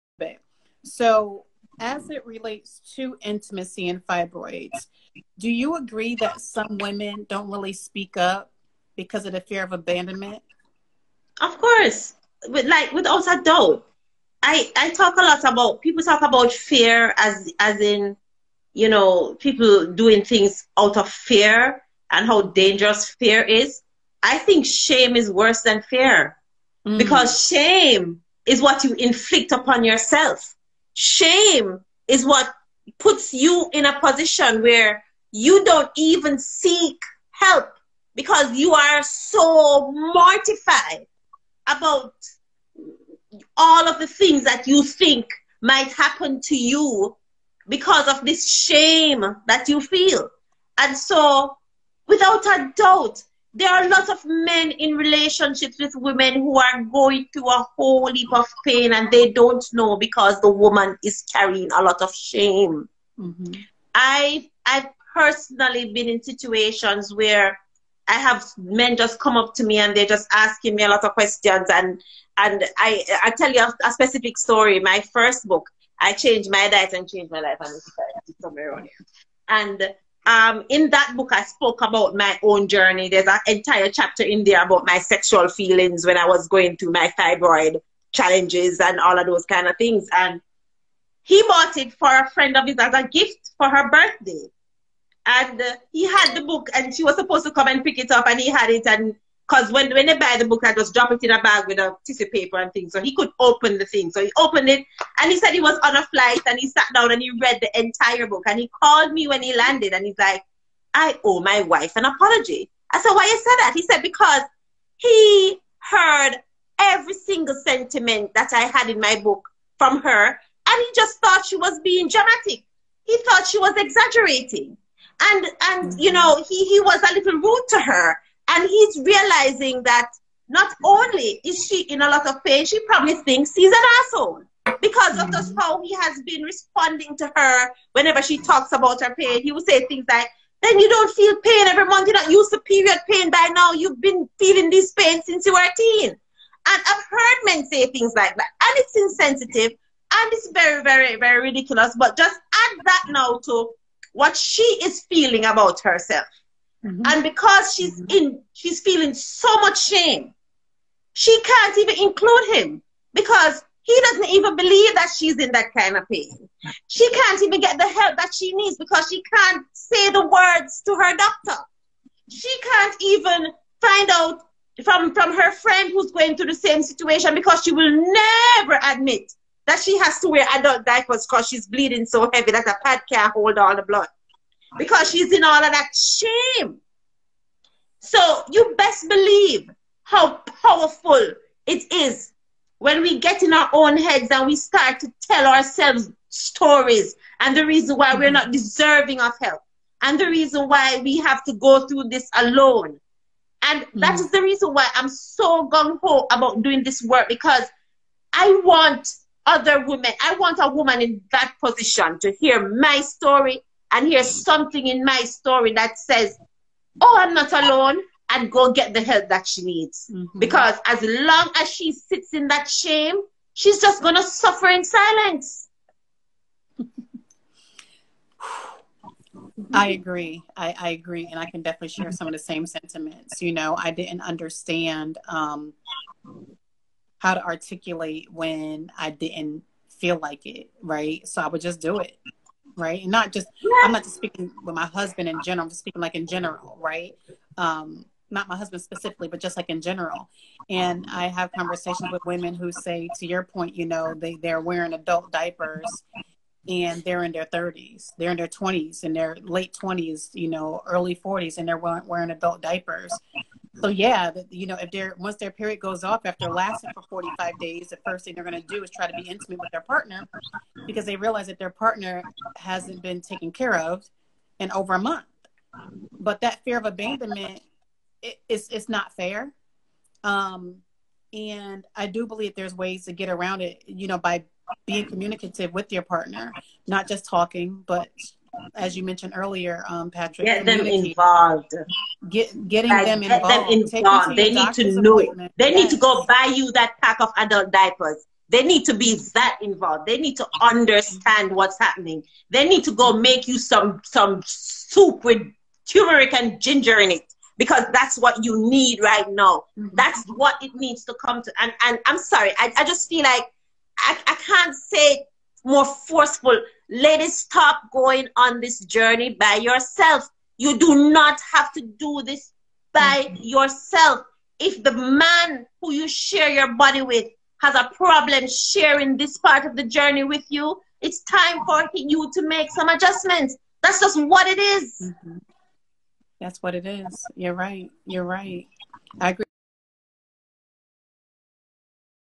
Fibroids Conversation with The Fibroid Pandemic Founder, Latoya Dwight
HerFlow Foundation's Founder and Executive Director, Shelly-Ann Weeks joins conversation about Fibroids on Instagram to talk about shame.